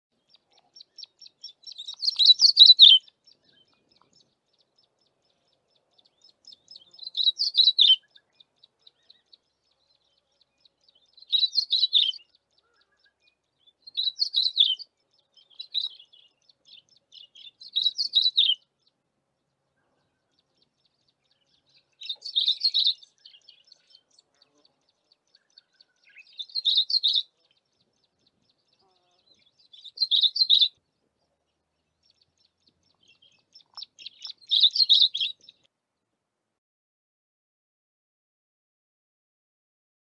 Yellow-bellied Eremomela singing
Listen to this little Yellow-bellied Eremomela singing while it was foraging for insects in a bush.